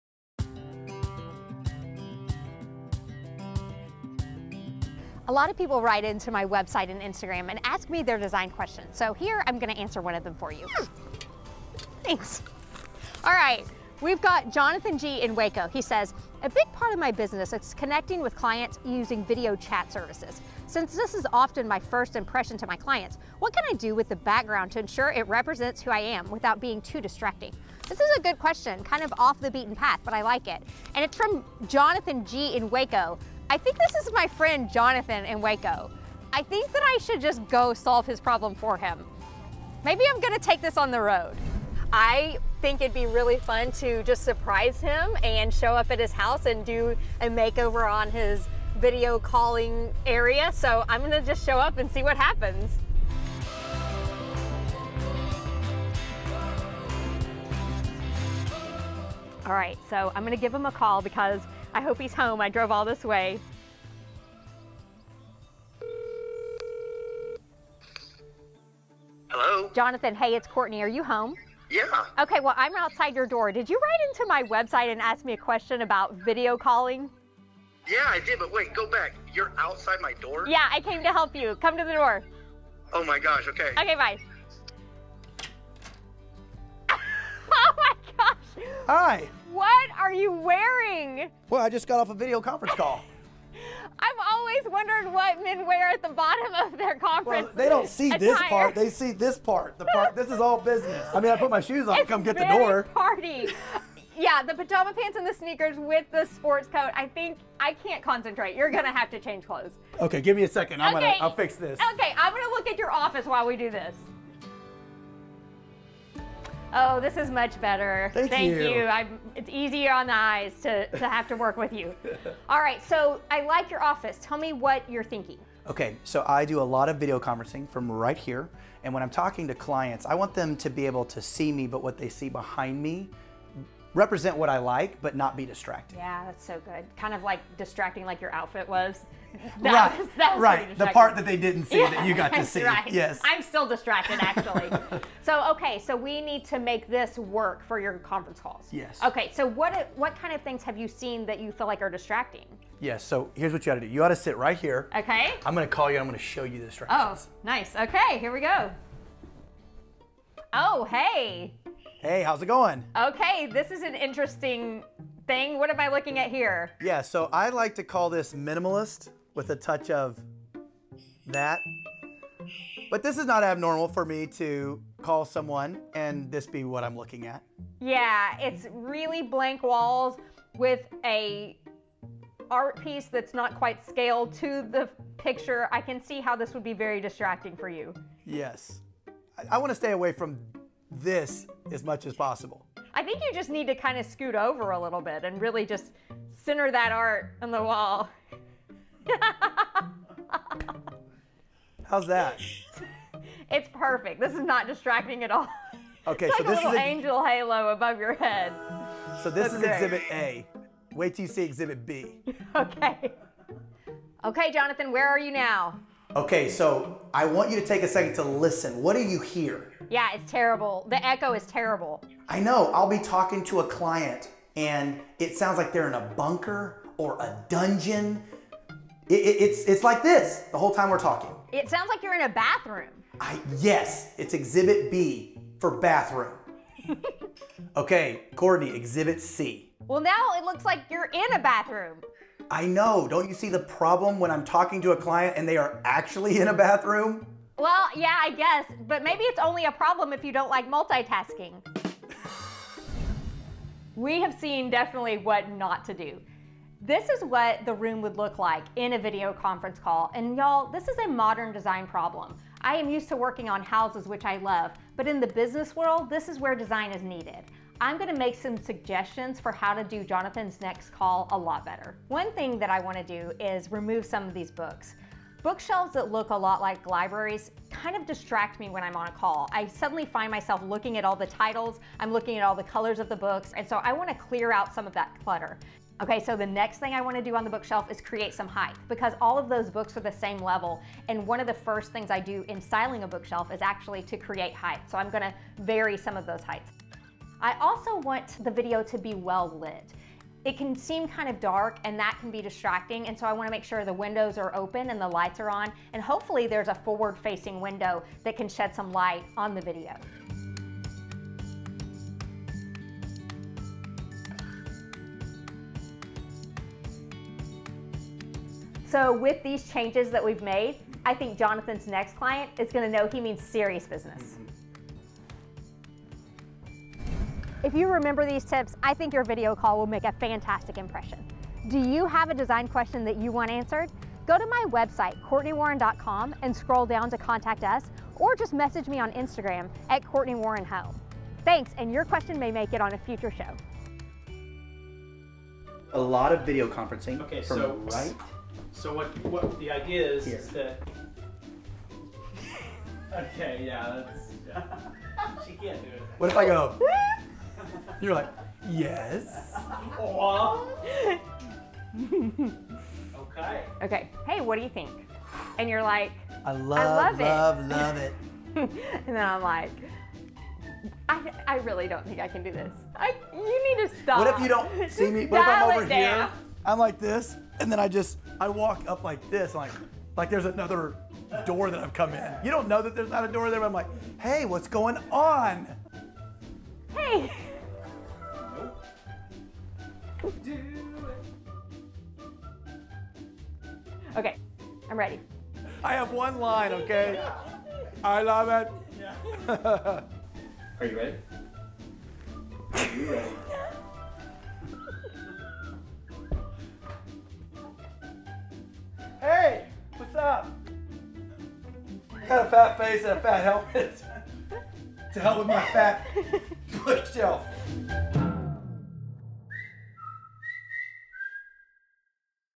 How to set up a Video/Zoom Call others WANT to watch
With so many viewers working from home at this time, designer Courtney Warren wants to help them improve their videos calls with more style and fewer distractions!

Tips & Visuals for Professional & Effective Video Conference Calls-